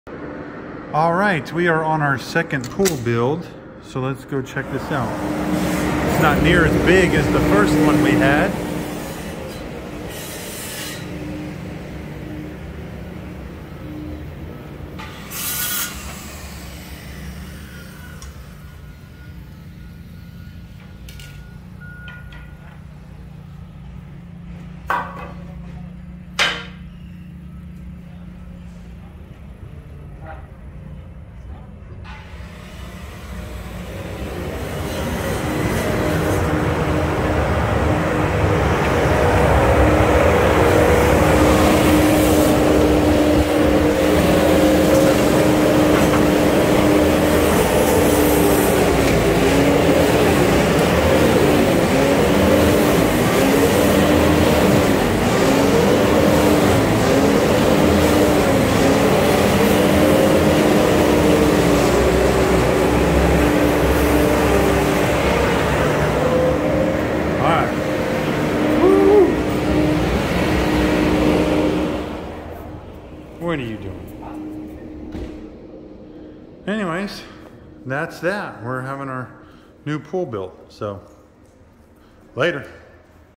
New pool build at our new house.
We sold our last home and moved into a smaller 4000 sq ft home.  The weird thing is and I never thought I'd even say it is that 4000 sq ft is much more manageable than almost 7000 sq ft.   We  made a quick $600k on the previous home, so now we are settling down for a few years in this area.  We just started our new pool build today, so we'll see how it turns out!!